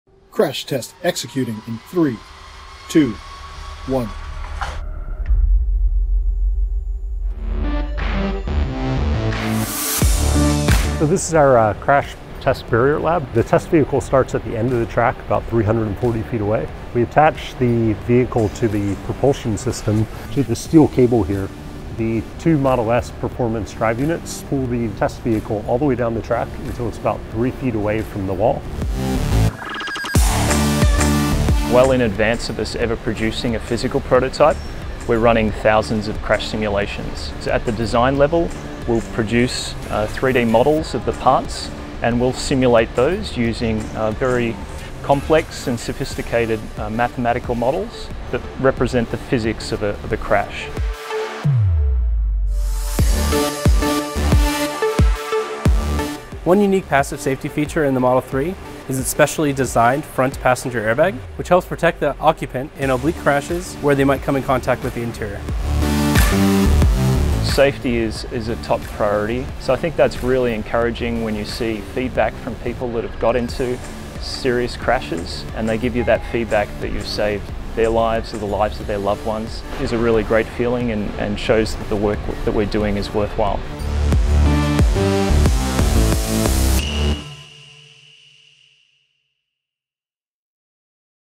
Tesla Crash Lab
Join us in the Tesla Crash Lab for a rare glimpse behind the scenes. The work our engineers do here is one of the reasons why Tesla vehicles are among the safest on the road.

The Model 3 has already earned a 5-star safety rating in every category and sub-category from safety authorities on three continents (North America, Europe and Australia), and it has received top marks around the world for its advanced safety assistance features like Automatic Emergency Braking. The Model 3 is also a 2019 IIHS TOP SAFETY PICK+ vehicle, the highest achievement awarded by the Institute.